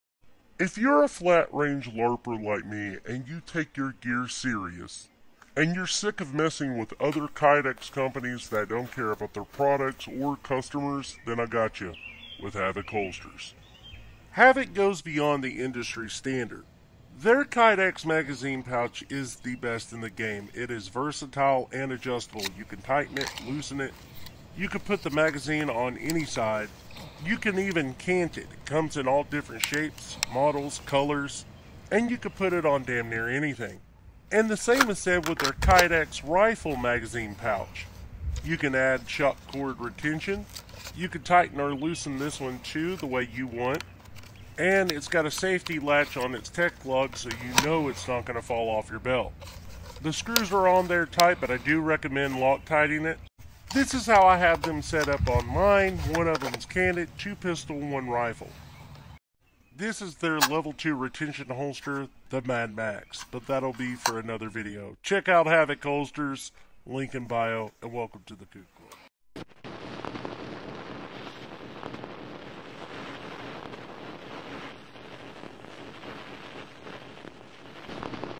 Havok holsters
There’s a lot of great kydex companies out there but in my opinion nobody does it like havok holsters

I’ve been doing a lot of shooting as of lately and testing out other gear but there’s just something about their products that works.

I wouldn’t lie to my followers or the homies for profit.

I have a closet full of holsters so when I say they are the best I mean it.

I’ve been testing out their stuff now for the last 6 months and this what I have found.

They are safe. My trigger on my gun isn’t exposed unlike others that always seem to not have a full trigger guard.

My MR920 fits like a glove and theirs room for an optic.

I don’t have to worry about any snags like a long sweat guard.

The thumb drive on the mad max is fast as Fuk boi.

When you push down the thumb drive you know it’s always going down. Not once has it failed on me.

It’s not noisy when I run. You don’t have any squeaky noises.

The mag pouches/carriers fits damn near any mag I’ve put in them. Mine are for Glock mags but others have worked and he makes them for all kinds of guns and magazines.

They aren’t super tight or loose. I can adjust them however I want.

They fit on my belt perfect✅ some kydex pouches you have to have a slim line belt in order for it to work. No with these.

Literally the only problems I have found is all user errors.

So in summary if you looking for a kydex company that is for the community. That actually wears their own products.

That is a family owned and operated small business right in America that cares about the customers & the product they send out.

@havokholsters is the kydex company for you.

Support yourself and the kookclub by supporting HH.